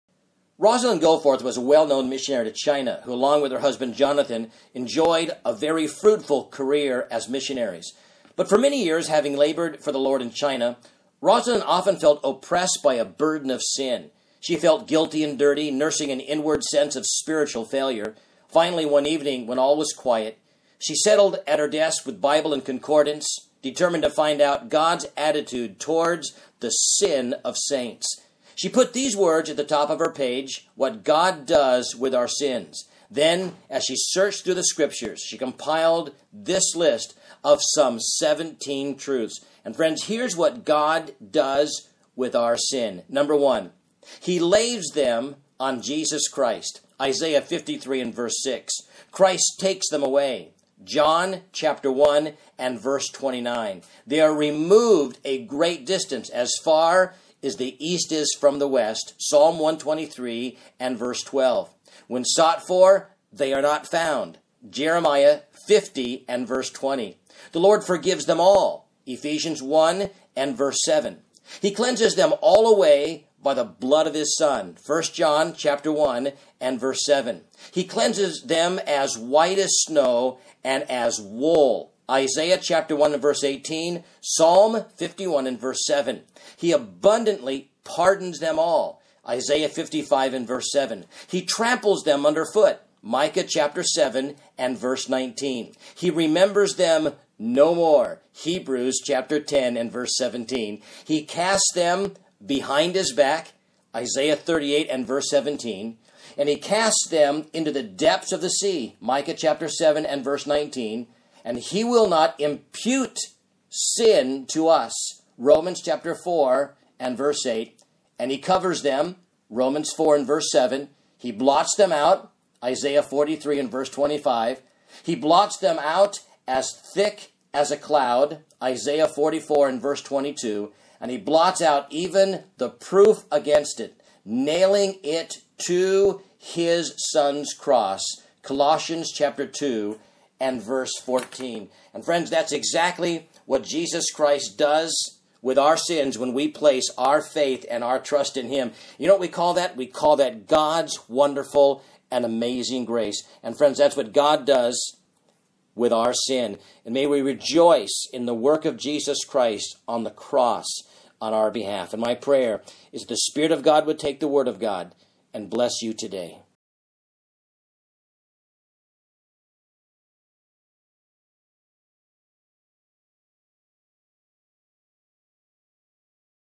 What God Does With Our Sin
Rosalind Goforth was a well-known missionary to China.  Along with her husband, Jonathan, enjoyed a very fruitful career as missionaries.  For many years having labored for the Lord in China,  Rosalind often felt oppressed by a burden of sins.  She felt guilty and dirty, nursing an inward sense of spiritual failure.  Finally, one evening when all was quiet, she sat down at her desk with Bible and Concordance, determined to find God's attitude toward the sin of Saints.  She put these words at the top of the page.  "WHAT GOD DOES WITH OUR SINS? " Then, as she searched through the scriptures, she compiled these lists of some 17  truths.  Friends, here's what God does with our sins :

1.  He lays them on Jesus Christ. Isaiah 53:6

2. Christ takes them away. John 1:29

3. They are removed a great distance , as far as the east is from the west. Psalm 103:12

4. When sought for, they are not found. Jeremiah 50:20

5. The Lord forgives them all. Ephesians. 1:7

6. He cleanses them ALL away by the blood of His Son. I John 1:7

7. He cleanses them as white as snow and as wool. Isaiah 1:18, Psalm 51:7

8. He abundantly pardons them all. Isaiah 55:7

9. He tramples them underfoot. Micah 7:19

10. He remembers them no more. Hebrew 10:17

11. He casts them behind His back. Isaiah  38:17

12. He casts them into the depths of the sea. Micah 7:19

13. He will not impute sins to us. Roman 4:8

14. He covers them. Roman 4:7

15. He blots them out. Isaiah 43:25

16. He blots them out as thick as a cloud. Isaiah 44:22

17. He blots out even the proof against it, NAILING IT TO HIS SON'S CROSS. Colossians 2:14

Friends, that's exactly what Jesus Christ does with our sins when we placed our faith and our trust in him.  You know what we called that?  We call that God's wonderful and amazing grace.  Friends, that is what God does with our sins.  May we rejoice in the work of Jesus Christ on the cross, on our behalf.  It is my prayer that the Spirit of God would take the Word of God and bless you today.